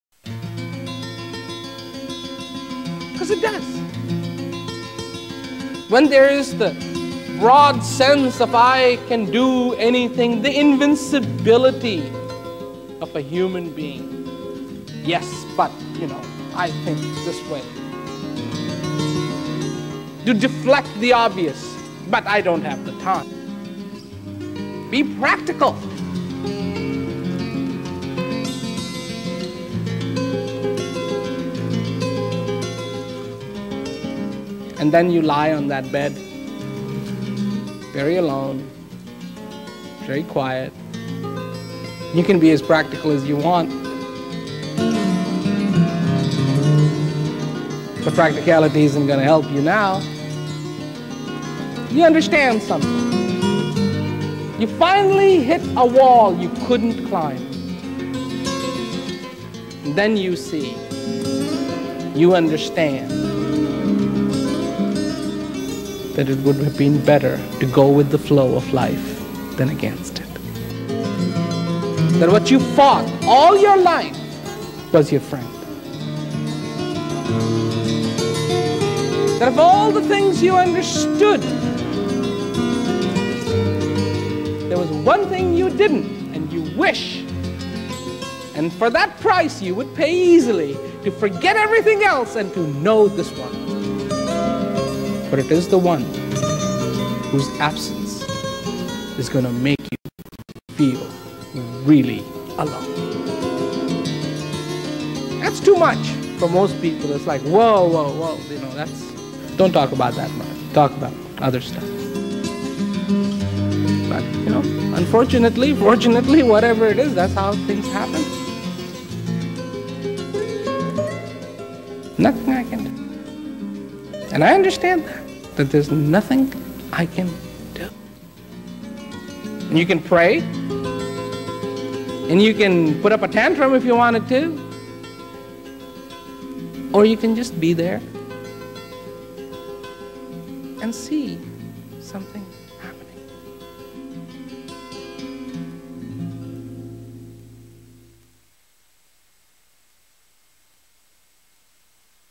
The Passing Bed Of Flowers
This is a comment on the importance of understanding life while we are alive.  Death comes always early. Wisdom comes always late.